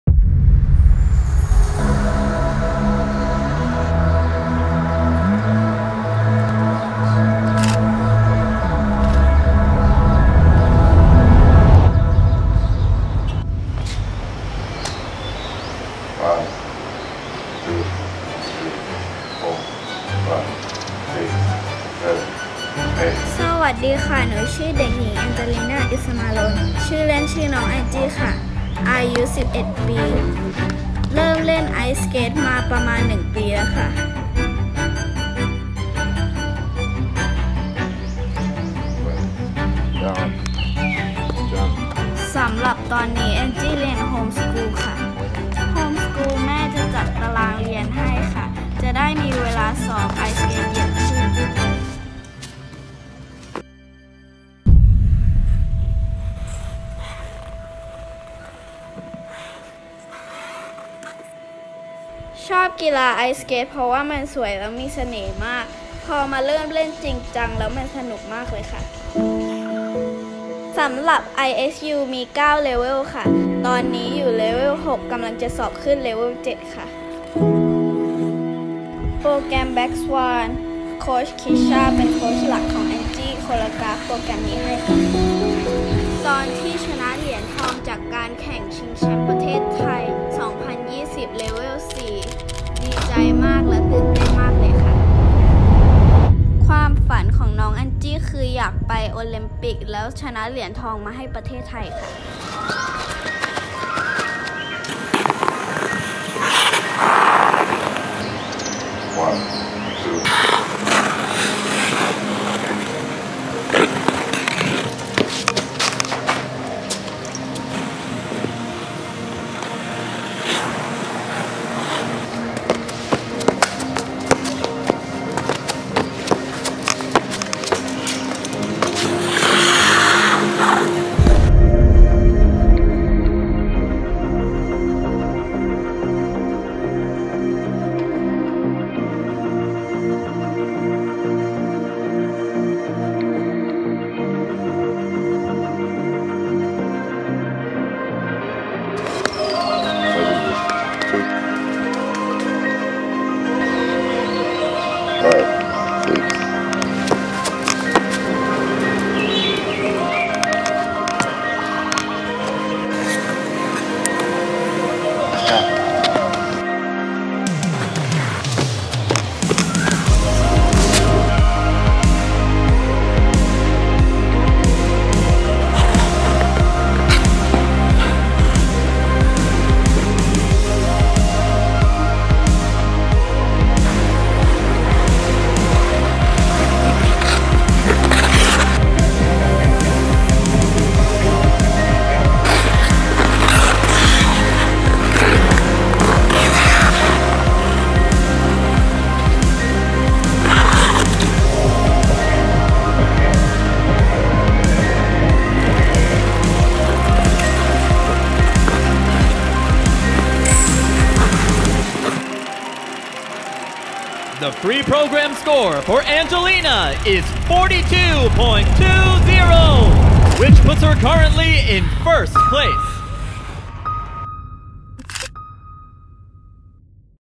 YOUNG SKATER "แองเจริน่า อิสมาโรน"
ตามฝันไปให้ถึงโอลิมปิกกับแองเจรีน่า อิสมาโรน สาวน้อยวัย 11 ปี นักกีฬาสเกตลีลา หรือ ฟิกเกอร์สเกตแชมป์ประเทศไทยปี 2020 (เลเวล 4) กับความรักในการเล่นสเกตและความมุ่งมั่นฝึกฝนเพื่อเส้นทางสู่ชัยชนะระดับโลกสายที่เธอเลือกเอง